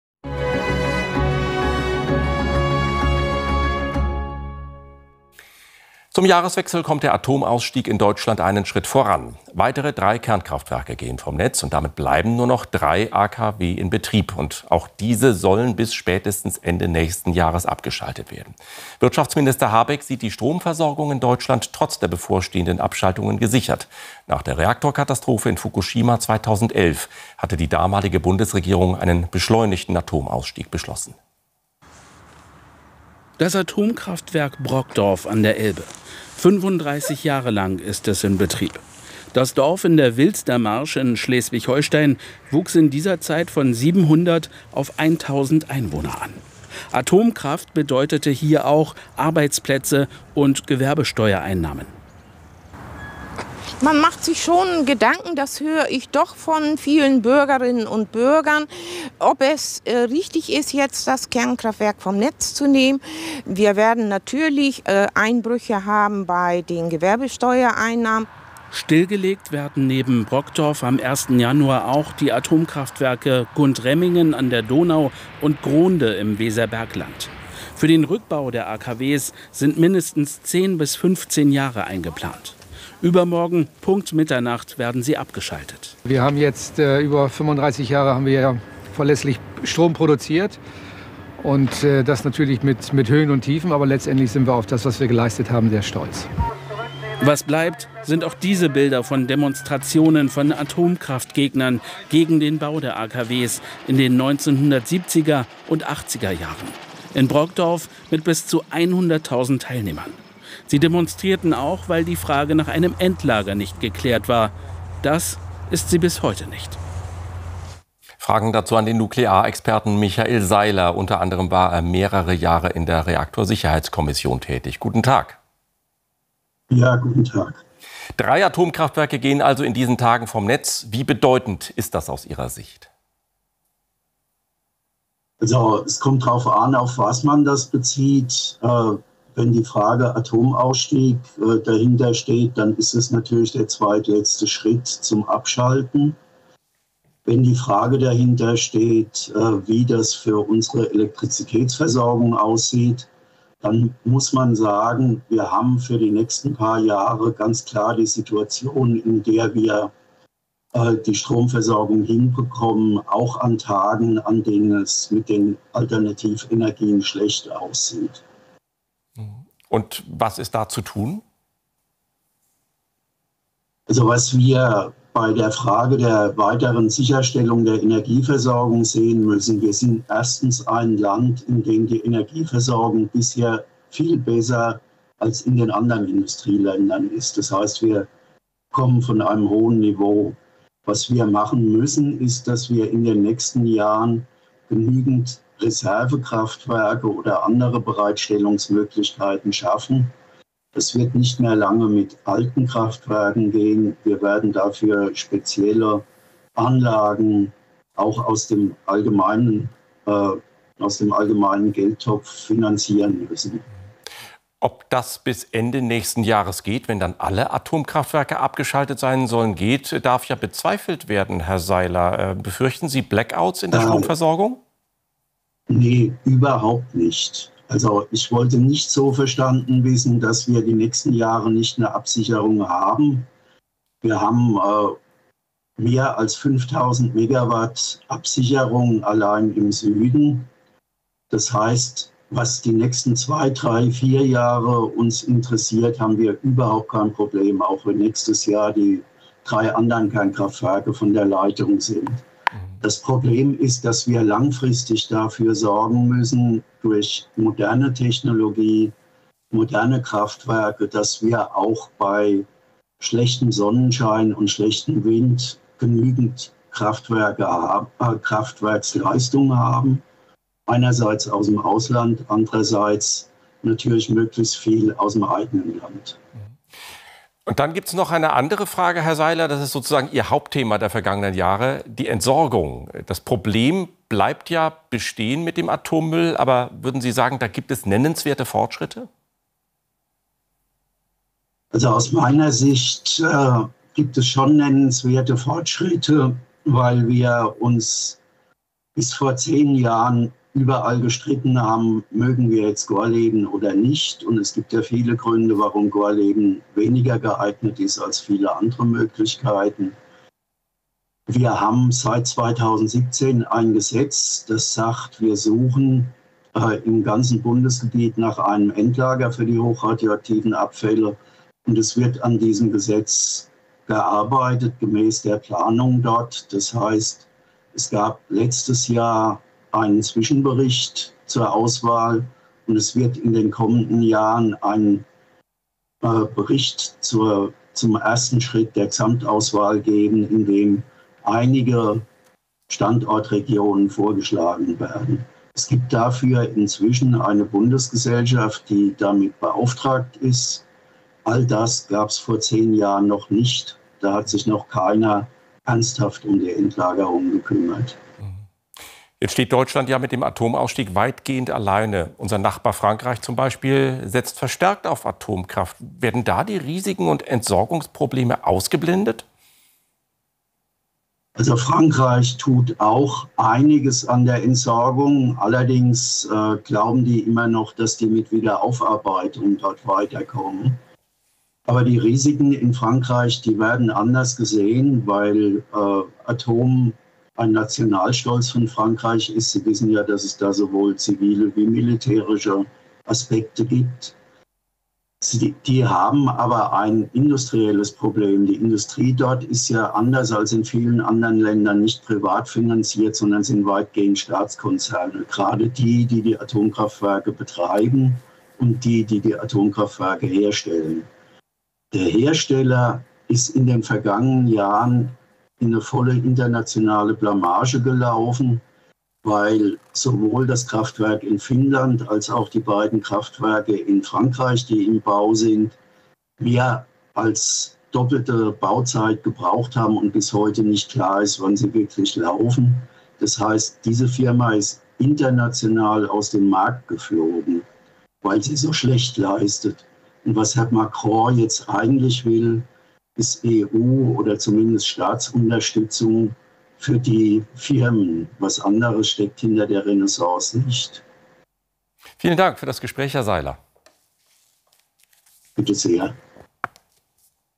Atomausstieg in Deutschland 👍 Drei Kernkraftwerke gehen vom Netz
Mit Ablauf des Jahres gehen weitere drei Kernkraftwerke vom Netz.
Drei Atomkraftwerke bleiben noch in Betrieb. Die Stromversorgung ist auch nach Abschaltung der Atomkraftwerke gesichert. Es sind die Kernkraftwerke Grohnde im Weserbergland, Brokdorf / Unterweser und Grundremmingen a.d.Donau.
In 2022 werden es die Atomkraftwerke Isar 2, Neckarwestheim und Emsland sein.

Für den Rückbau dieser AKW's wurde jeweils ein Zeitraum von mindestens zehn bis fünfzehn Jahren veranschlagt. Fragen nach einem Endlager sind bis heute nicht geklärt. Das war stets einer der Anstöße, die zu großen Protesten in den 70er und 80er Jahren führten. Nachhaltigkeit - Illusion oder Wirklichkeit Atomausstieg in Deutschland - Drei Kernkraftwerke gehen vom Netz. Unterwes |